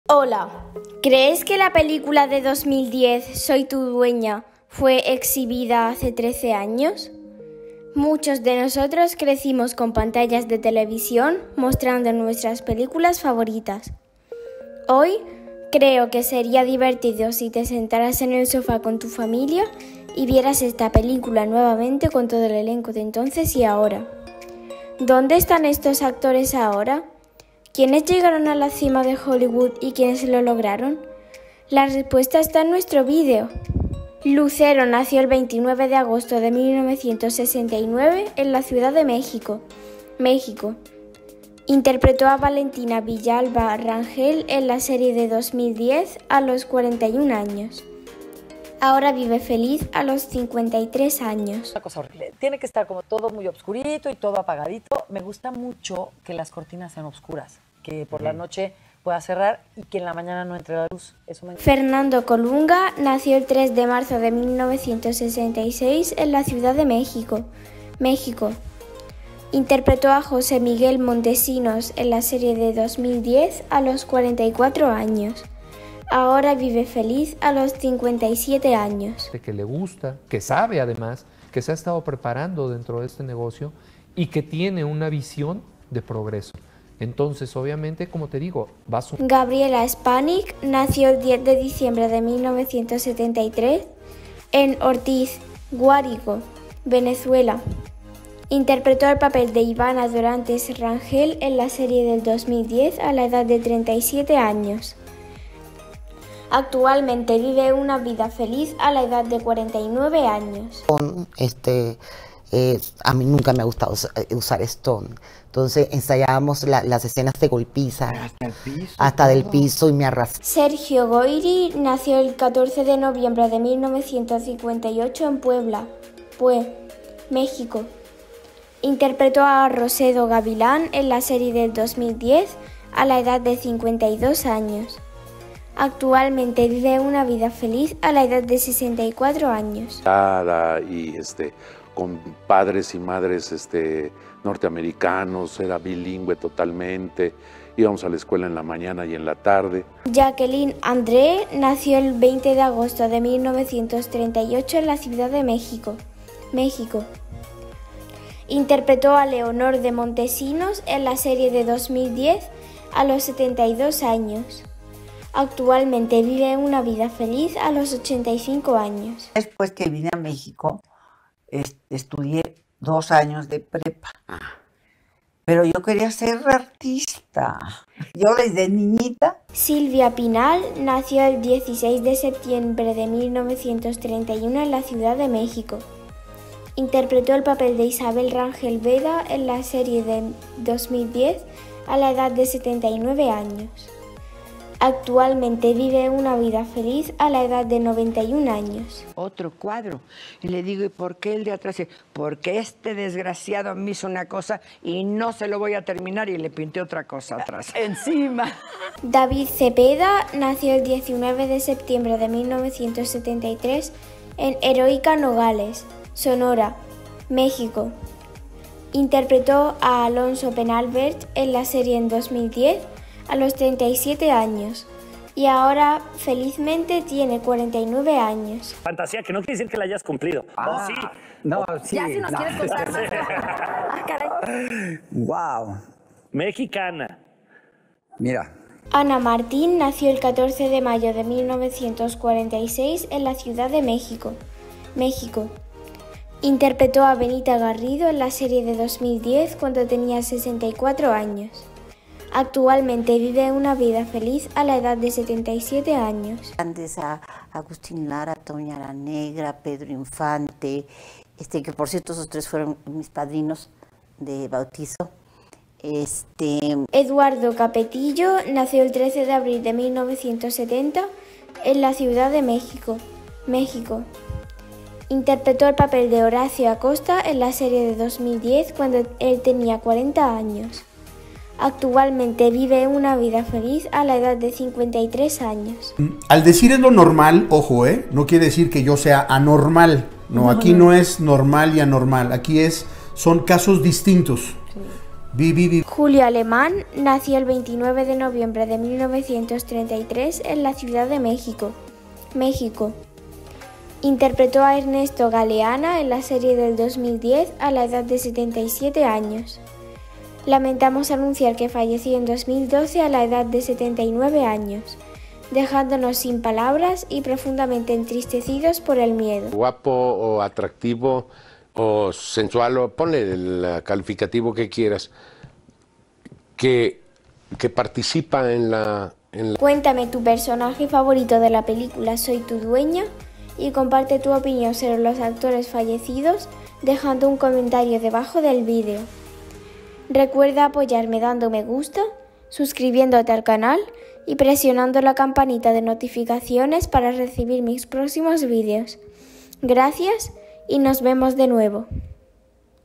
Soy Tu Dueña (2010) Elenco: Antes y ahora 2023, ¿Quién falleció después de 13 años?
Soy tu dueña es una telenovela mexicana producida por Nicandro Díaz González para Televisa, fue transmitida por el Canal de las Estrellas el 19 de abril de 2010. Fue una versión de la telenovela venezolana, La doña producida por RCTV en 1972 escrita por Inés Rodena, que a su vez Televisa realizó esta versión en 1995 con el nombre de La dueña.

Protagonizada por Lucero y Fernando Colunga, con las participaciones antagónicas de Gabriela Spanic, Sergio Goyri, David Zepeda y Jacqueline Andere; con las actuaciones estelares de Eduardo Capetillo, Marisol del Olmo y Julio Alemán, Ana Martín, Eric del Castillo y Silvia Pinal.